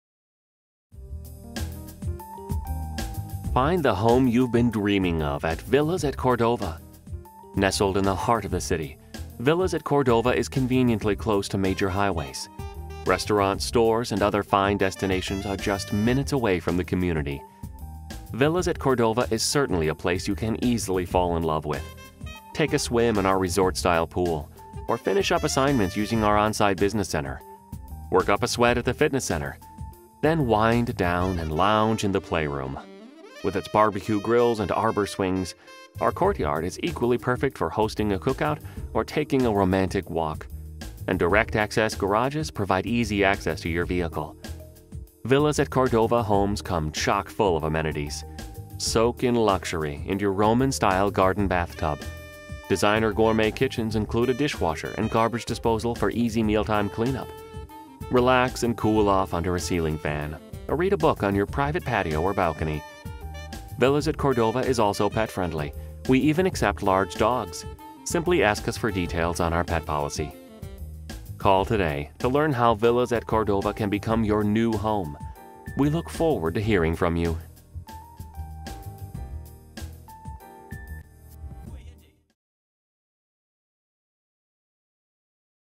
Villas at Cordova Apartments in Cordova, TN - ForRent.com
Villas at Cordova Apartments for rent in Cordova, TN on ForRent.com: Availability, pricing and special promotions subject to change daily. A world of luxury you will certainly want to call home.  We are a gated community with amenities such as a stunning resort style pool, resident business center, fitness center, and playroom.  Our homes also have gourmet kitchens, ceiling fans, and intrusion alarms.  We are in the heart of Cordova with easy access to Germantown Parkway and Interstate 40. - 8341 Macon Road